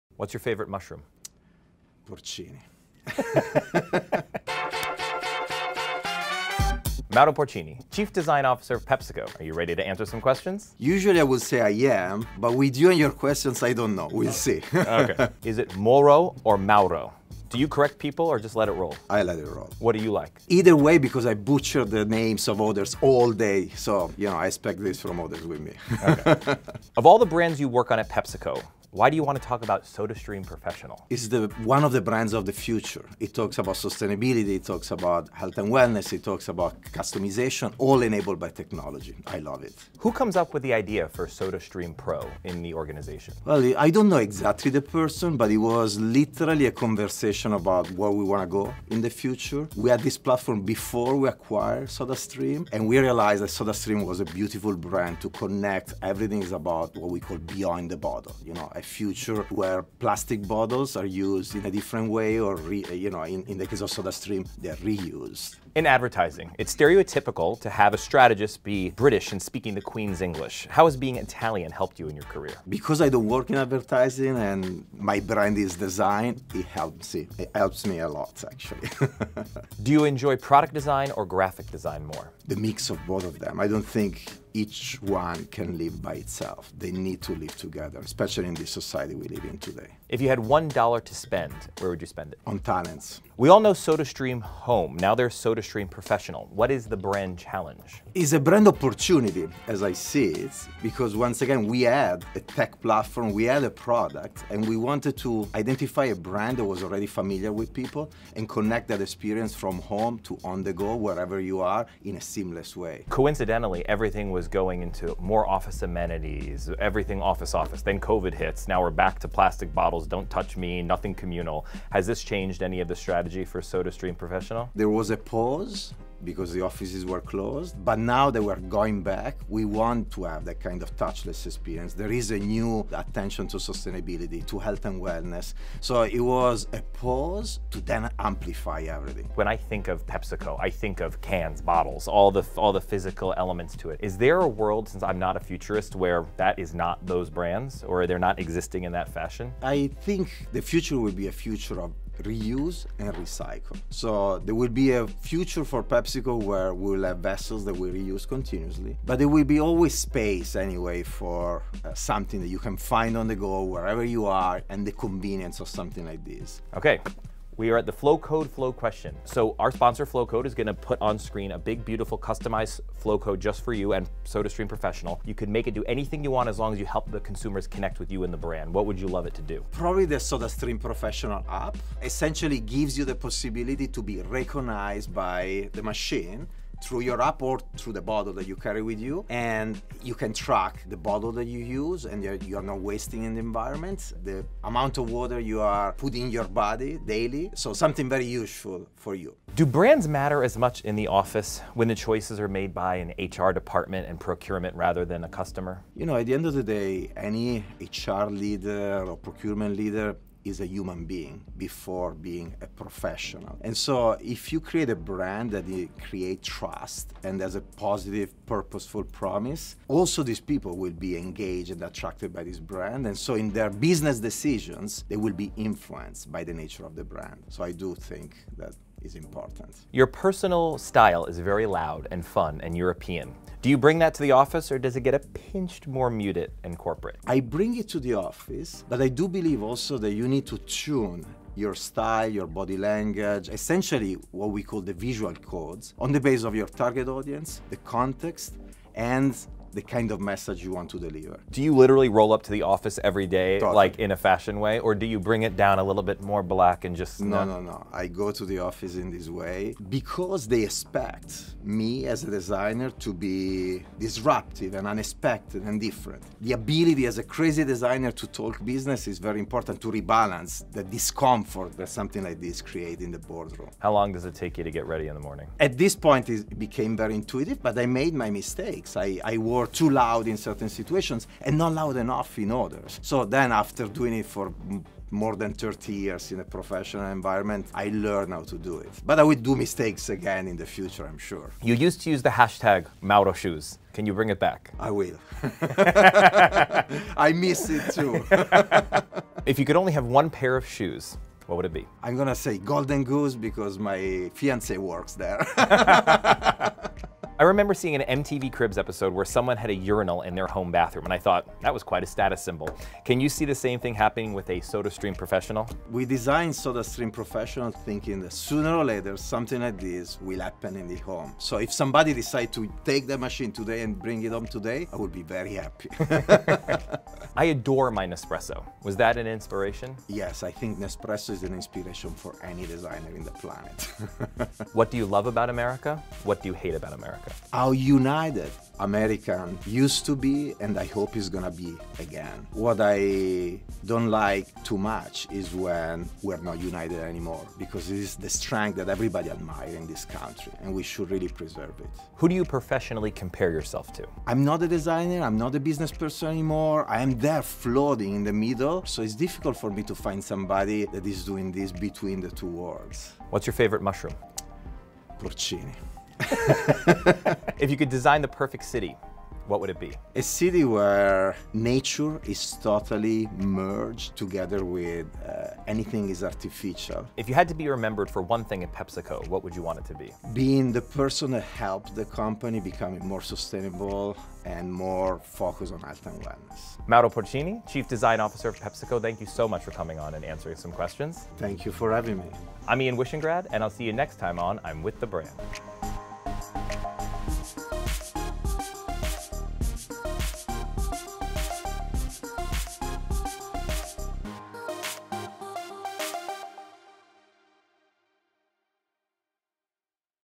I'm With The Brand | Design Veteran Mauro Porcini on the Future of Beverage
In his nearly 10 years at PepsiCo, chief design officer Mauro Porcini has had the opportunity to work on countless projects and brands. So why is it that SodaStream Professional excites him so much? “It’s one of the brands of the future,” Porcini says. “It talks about sustainability, it talks about health and wellness, it talks about customization, all enabled by technology.”

PepsiCo acquired SodaStream in 2018 — supporting and promoting the brand’s commitment to reducing plastic waste through reusable bottles that allow customers to make personalized, flavored sparkling beverages at home. SodaStream Professional is a fully connected ecosystem including a dispenser, a bottle, and an app, and is meant for offices, colleges, and airports (though Porcini certainly wouldn’t mind if someone put one in their home).

Hear all about how Porcini and his team approached SodaStream Professional on this week’s episode of I’m With The Brand.